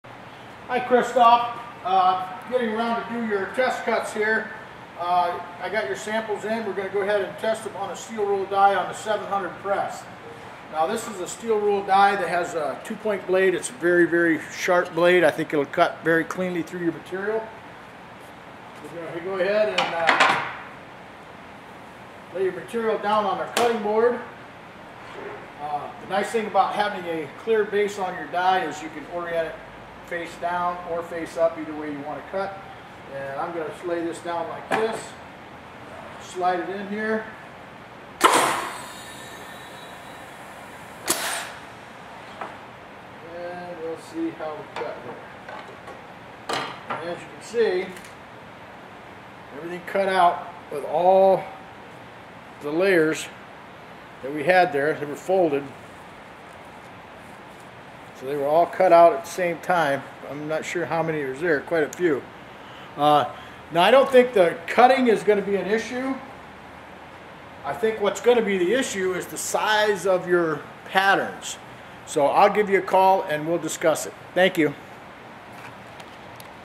Clicker 700 cutting press cutting thin plastic using a .937 steel rule die
Don demonstrates the Tippmann Clicker 700 steel rule die cutting press cutting thin plastic using a .937 steel rule cutting die